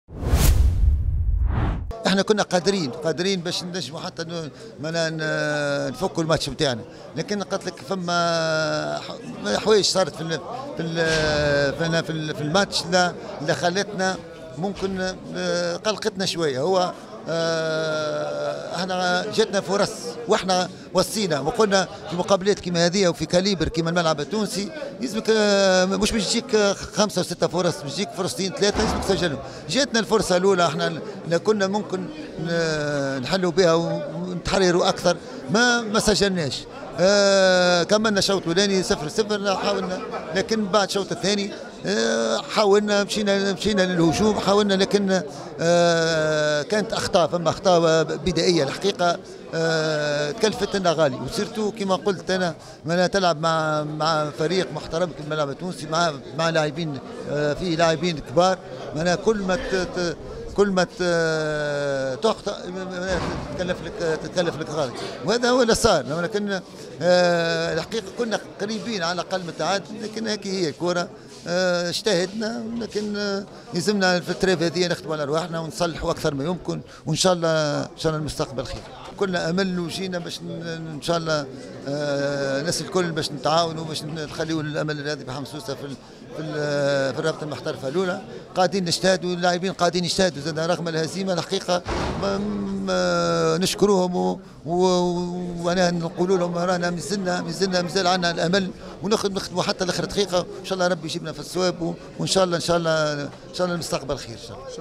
!مدرب أمل حمام سوسة : تلعب مع فريق كبير كيفما الملعب التونسي.. كل ما تخطئ تتكلفلك غالي
مدرب أمل حمام سوسة عبد الحي العتيري: تلعب مع فريق كبير كيفما الملعب التونسي.. كل ما تخطئ تتكلفلك غالي!